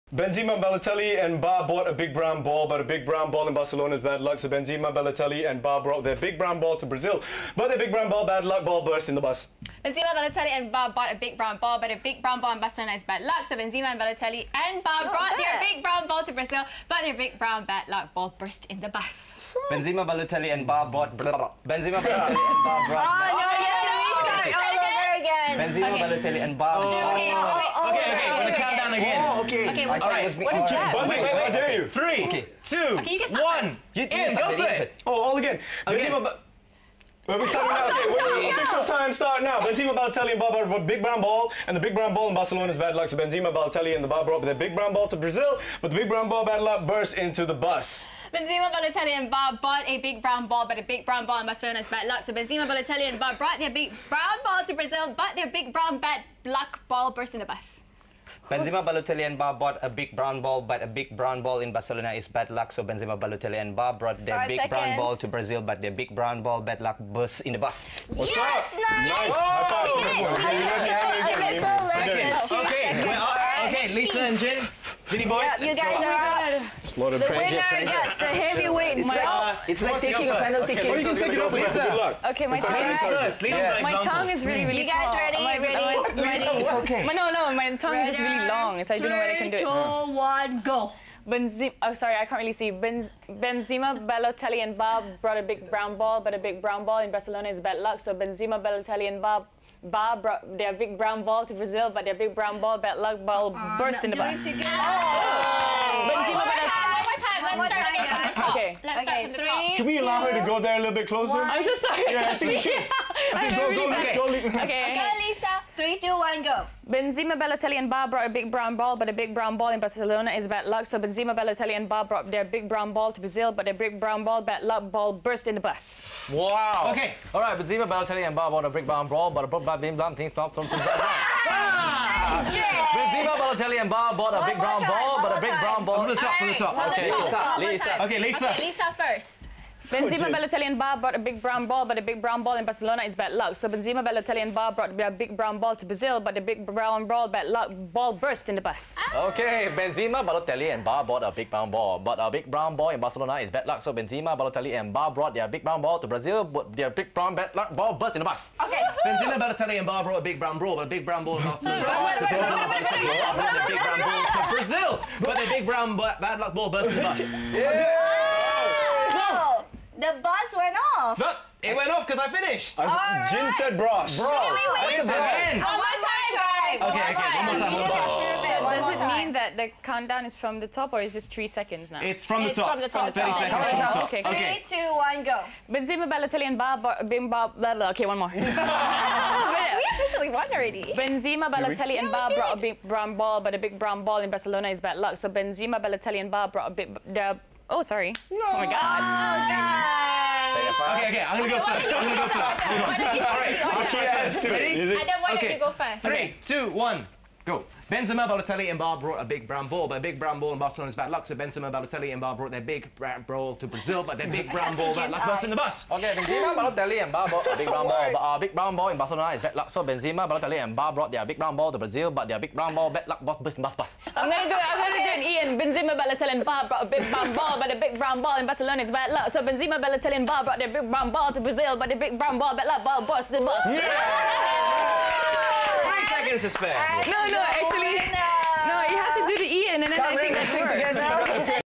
S4 E3: Twister with Lisa Surihani, Farish Aziz & Hitz.fm Morning Crew
Complete 3 football tongue twisters in the space of 30 seconds.